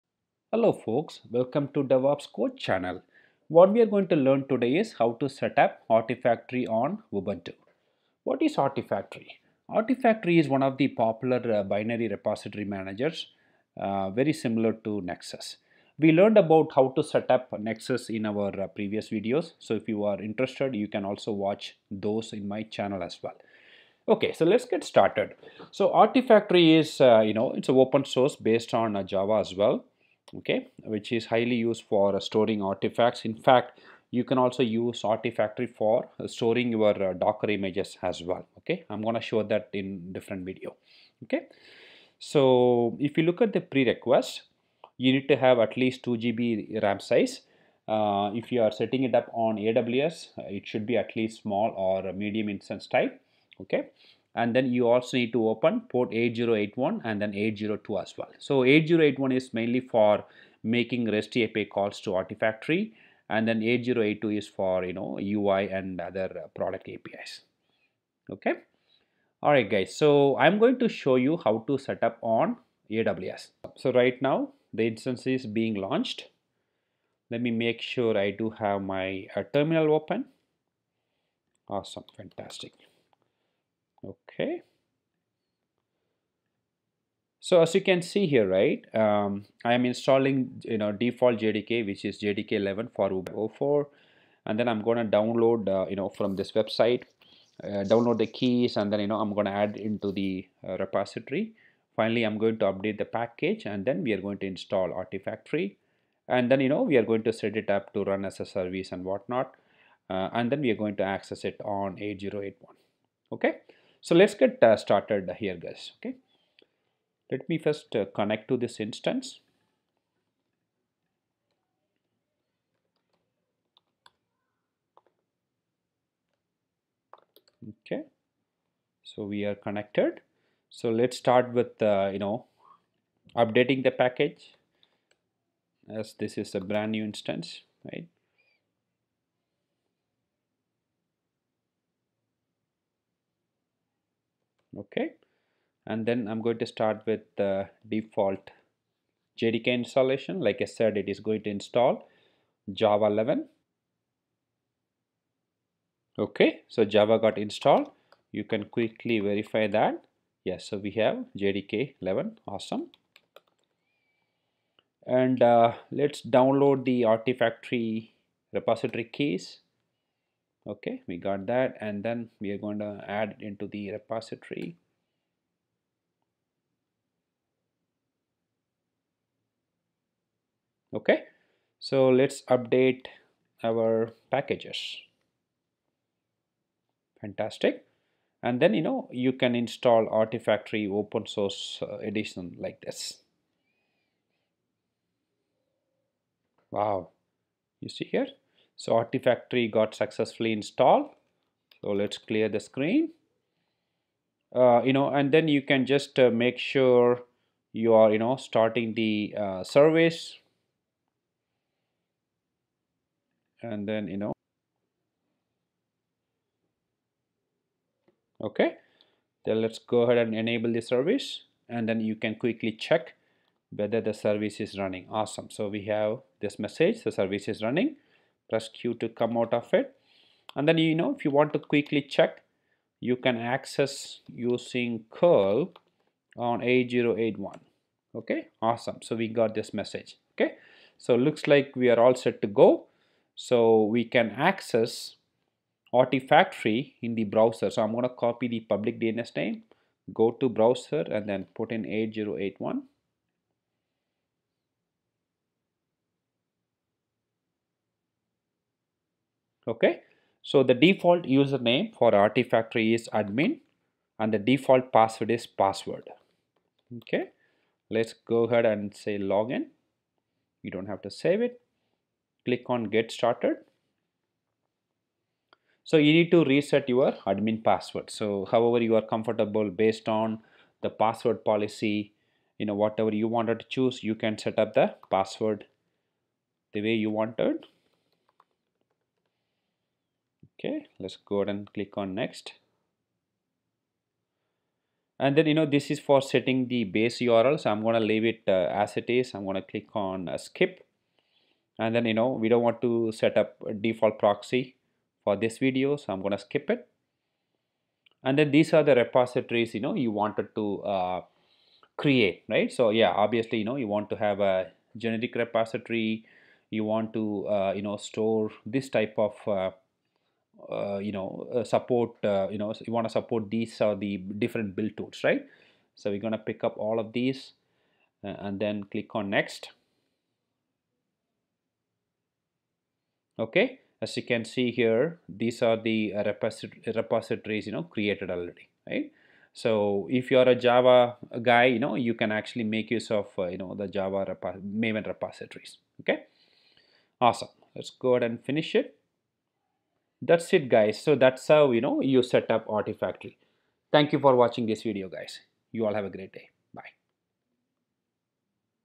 How to Install Artifactory on Ubuntu | Setup JFrog Artifactory on Ubuntu Server
Learn how to install and set up Artifactory on Ubuntu in this step-by-step DevOps tutorial. Artifactory is a popular Java-based binary repository manager used for storing artifacts, including Docker images and build dependencies.

In this video, you will learn:
What Artifactory is and why it’s important for DevOps workflows
System prerequisites for installing Artifactory on Ubuntu
Installing Java JDK 11 as a dependency
Adding the Artifactory repository and installing the open-source edition
Starting, enabling, and verifying the Artifactory service
Accessing Artifactory via browser and initial setup (admin user, password reset, repository configuration)

Pre-requisites:
Ubuntu instance (local or AWS EC2)
Minimum 2 GB RAM
Open ports 8081 (REST API) and 8082 (UI/API access)
Basic knowledge of Linux terminal commands